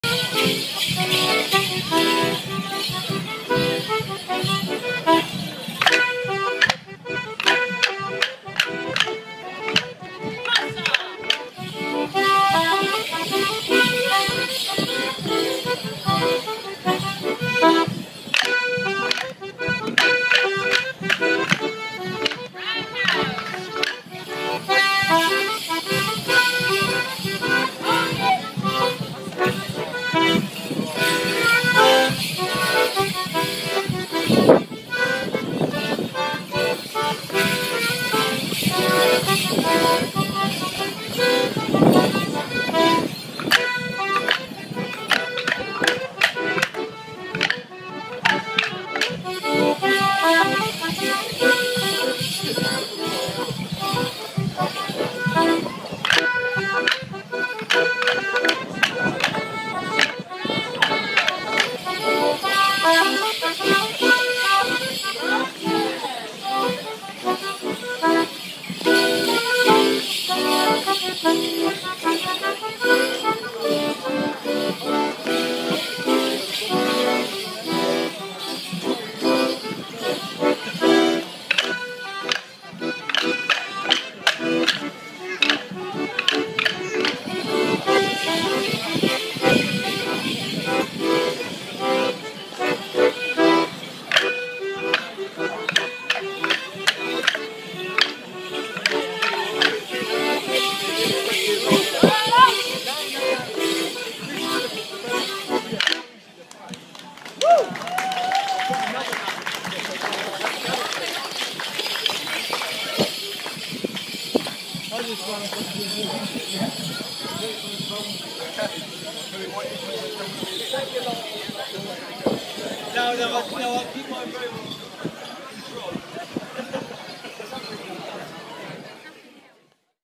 SHERINGHAM POTTY MORRIS DANCING 2013.
The Annual Potty Morris Dancing Event held in Sheringham, North Norfolk. 2013.                  Filmed and edited by Joi Joi and Actionmoviecromer.